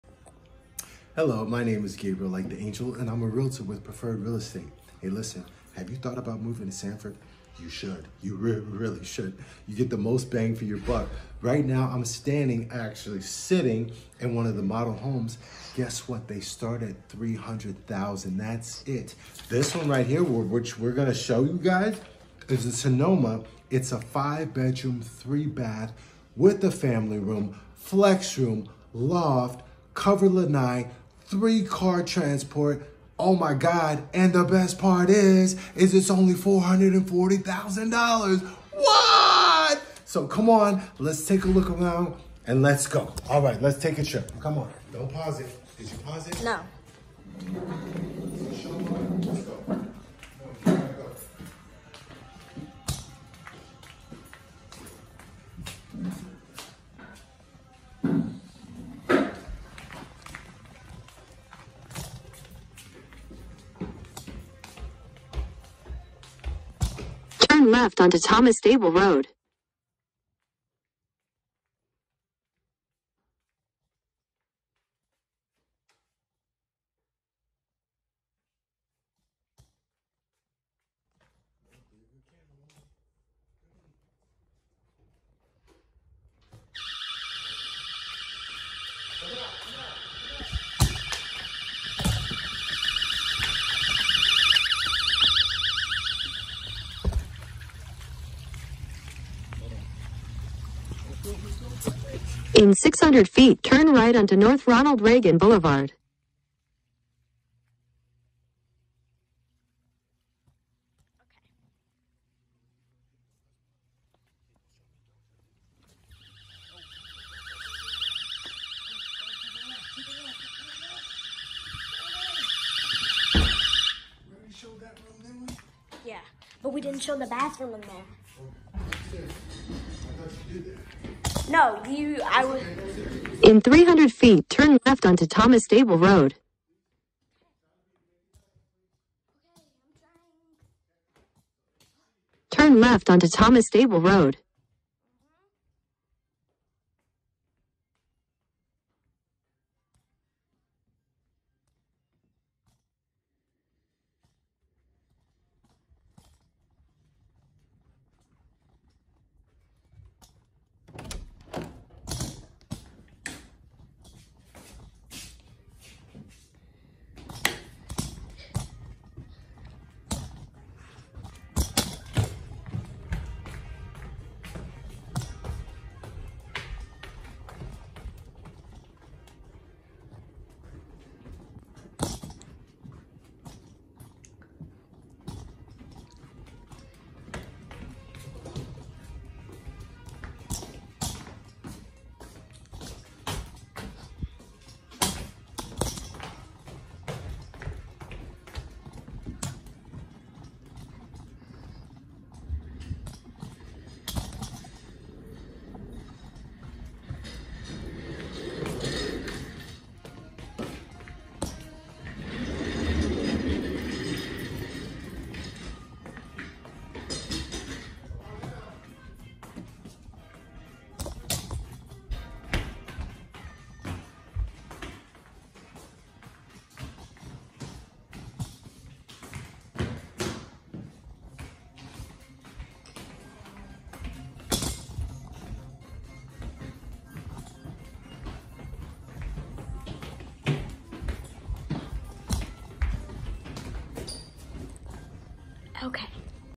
Real estate: New home construction in Sanford Florida Florida￼
So Since my mobility isn’t the best I have my mini real estate assistant help me shoot the videos.

If you have been thinking about moving to Sanford now is the time. This is beautiful new construction and the homes start at $300,000. If you would like to place an offer call me as he’s won’t last long!

Gabriel “Like The Angel” Haynes`